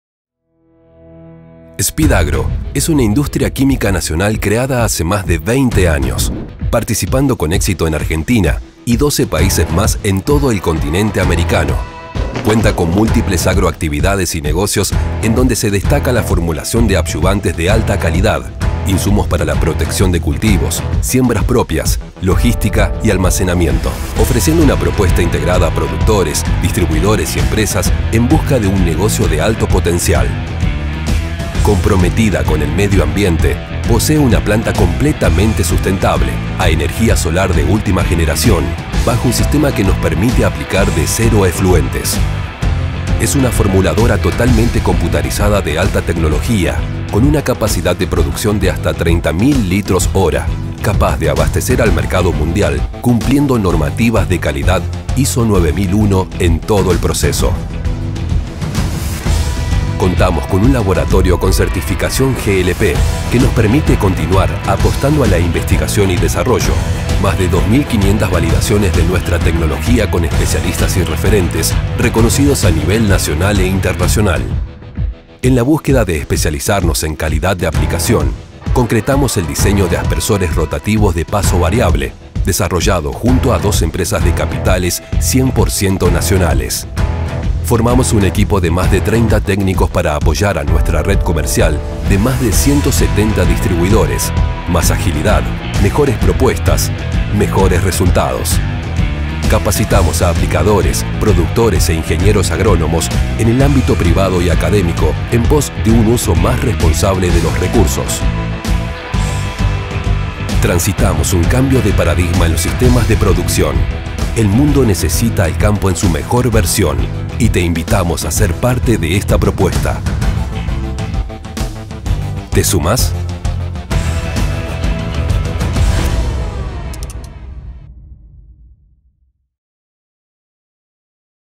MBTV || Institucional SPEEDAGRO 2022
Video institucional para SPEEDAGRO, realización integral de nuestra productora Martín Bustamante Imagen & Comunicación